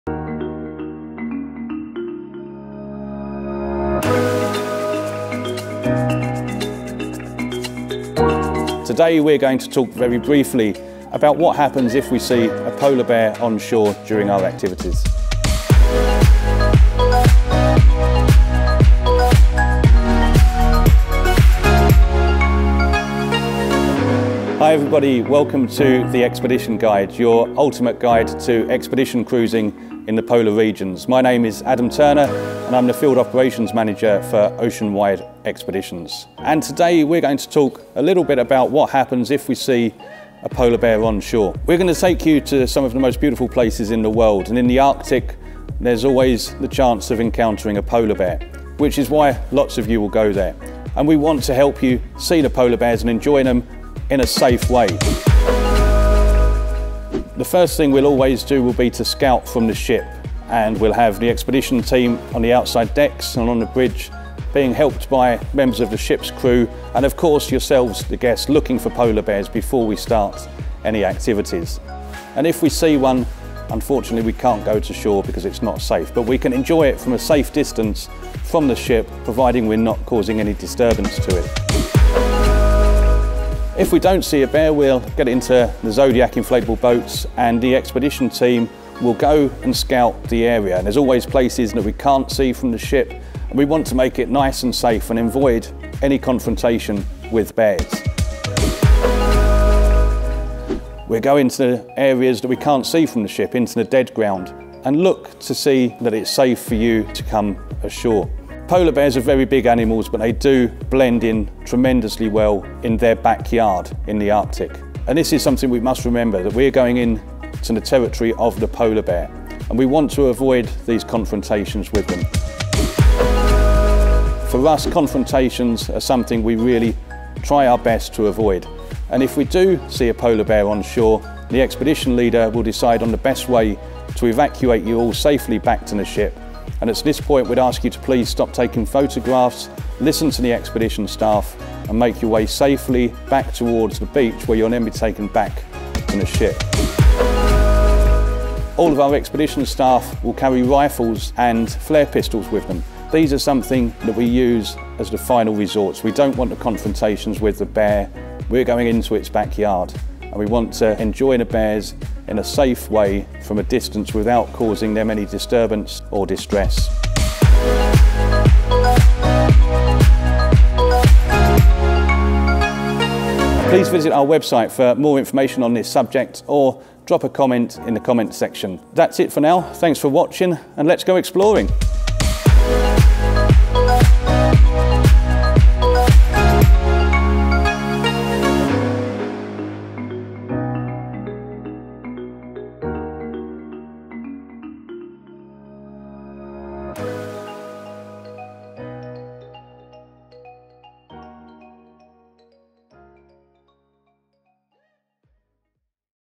Polar Bear Safety
How not to get eaten by a polar bear
Humor aside, polar bears can pose a real safety concern to travelers in certain areas of the Arctic. This is why we follow strict safety protocol during all expeditions there. Though seeing a polar is a big part of why people visit the Arctic, the goal is not to end up inside of one. For this reason, we’ve made this informative polar bear safety video for returning passengers and newcomers alike.

In this video, our field operations manager, Adam Turner, demonstrates the extensive precautions we take to avoid polar bear confrontations. A veteran expedition leader with years of experience guiding travelers safely into and out of Antarctica and the Arctic, Turner’s tips comprise the core safety “commandments” when it comes to our operations in polar bear country.

These precautions include:

1. Scouting from the ship
Before any shore landing can be made, our expedition team (guides, crew, even the captain) pre-sight our intended landing area. These efforts are also aided by the passengers themselves. If any polar bears are seen on shore, we cannot land. But that does not mean we can’t enjoy watching the bear or bears from a safe distance, and in fact some of our best bear photos are captured this way.

2. Scouting from the Zodiac boats
Once we’re satisfied that our initial ship-to-shore search has revealed no bears, our guides will load into the Zodiac boats (which we use to deliver passengers to and from land) and do a closer search of the intended landing site. There are always areas we cannot fully see from the ship, so this additional precaution helps make sure no bears have escaped notice.

3. Scouting from the land
But because polar bears are evolutionarily designed to avoid detection in their natural habitat, preliminary scouting from the ship and Zodiacs is not enough to ensure that our intended landing site is bear-free. We must therefore get onto dry land for an even closer look, making sure that the environment is as safe as possible for our passengers.

4. Evacuating in the event of a sighting
Even when using the most stringent safety protocol, it’s impossible to completely eliminate the possibility of a polar bear encounter. In the exceedingly rare event a polar bear is spotted once we’re on shore, our guides will determine the best evacuation strategy. Passengers must then stop taking photos and follow all guide instructions for a safe and speedy escape.

5. Using safety flares and bear rifles as a last resort
In the Arctic, all our expedition guides carry flare pistols and bear rifles when on shore. We hope they never have to use them, but in the unlikely event a polar bear surprises our staff, these tools represent the final resort in preventing an attack. Since we recognize that we’re the visitors, we always exhaust all other possibilities before using flares or rifles.